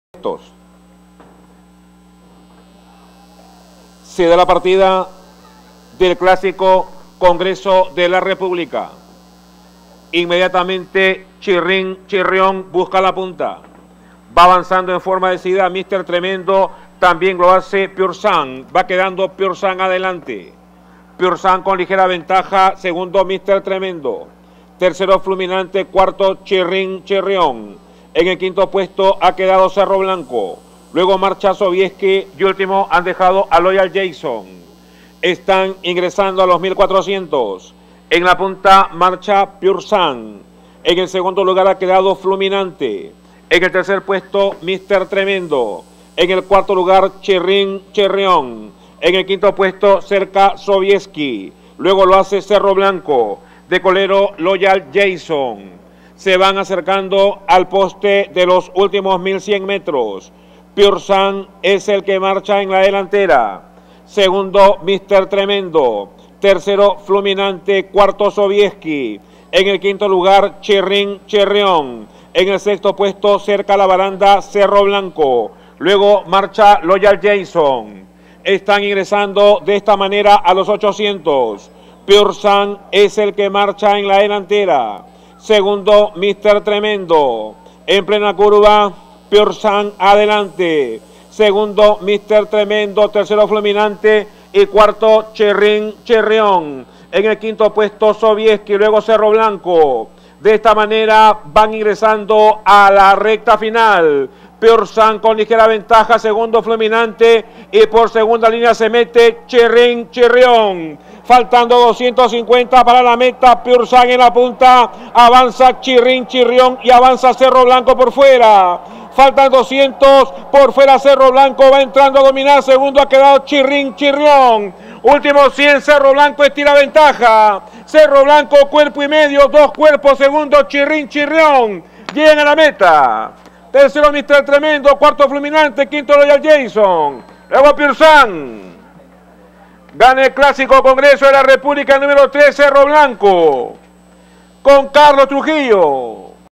CERRO BLANCO - Clásico Congreso de la República – 2,000 Mts.
Cerro Blanco (Curlin) remató en gran forma para vencer en el Clásico Congreso de la República, una competencia para todo caballo no ganador de clásico en la temporada, a peso por edad, que se disputó el domingo 19 de octubre sobre 2,000 metros en el Hipódromo de Monterrico. El pensionista del Stud Agustín Gabriel, nacido en USA, derrotó por 4 ¾ cuerpos a Chirrin Chirrion (Flanders Fields). Tercero a 5 ½ cuerpos lo hizo Mr. Tremendo (Henny Hughes) y cuarto a 8 ½ cuerpos finalizó Fluminante (Privately Held).

Desarrollo

Pur Sang fue el puntero seguido por Mr. Tremendo, Fluminante, Chirrin Chirrion, Cerro Blanco, Sobieki y Loyal Jason.

Llegando al kilómetro final, Pur Sang comandaba al conjunto, mientras en su búsqueda iban Mr. Tremendo, Fluminante, Chirrin Chirrion, Cerro Blanco, Sobieki y Loyal Jason.

Ingresando a la recta final, aflojó Pur Sang cuando Chirriin Chirrion asomaba por segunda línea y por el lado exterior aparecía Cerro Blanco.

En los 200 finales, Cerro Blanco siguió bajando para pasar de largo. Segundo quedó Chirrin Chirrion, mientras Mr. Tremendo y Fluminante completaron el marcador. Loyal Jason, Pur Sang y Sobieski arribaron luego.

Antecedentes

Cerro Blanco es un tordillo de 5 años, por Curlin y Alexine (Runaway Groom), nacido en USA y que pertenece al Stud Agustín Gabriel. Eduardo Pianezzi II lo presentó en su mejor estado y en sus controles estuvo impecable el líder Carlos Trujillo. Este fue el 6to. triunfo en 16 salidas para Cerro Blanco que totaliza hasta el momento S/. 128,653 en premios. El registro: 2´08”64.